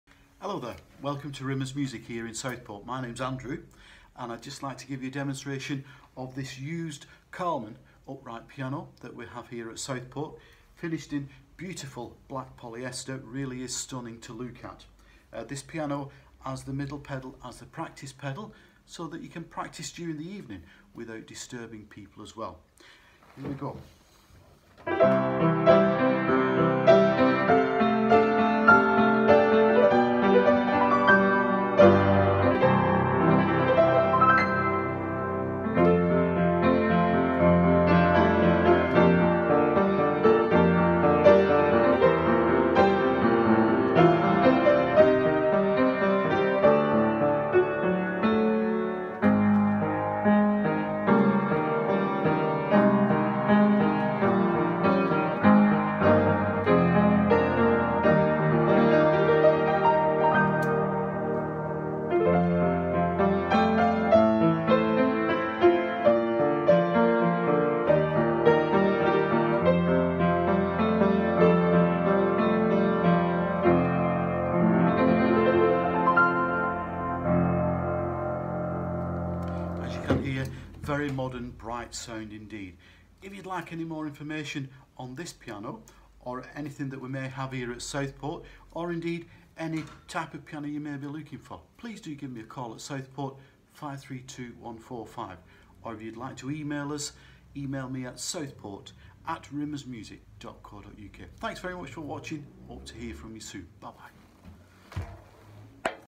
Carlmann Upright Acoustic Piano | Used | Rimmers Music
Here we have Andrew at Rimmers Music Southport Store showing you the Carlmann Upright Acoustic Piano | Used.

Carlmann Upright Acoustic Piano | Used
Our Carlmann 110 upright piano is a modern cabinet design , a nice compact model that would fit into most homes. Finished in very nice high gloss polished black , this model has three pedals , the middle one being a practice pedal for quiet practice in the evening times. The overall dimensions are 110cm high , width 145cm and a depth of 57cm. The piano has a bright vibrant tone characteristic of this size of piano , the action is a medium feel and is very nice to play.
Andrew - Rimmers Music Southport Store

You can find out more about this and other pianos on Rimmers Music website.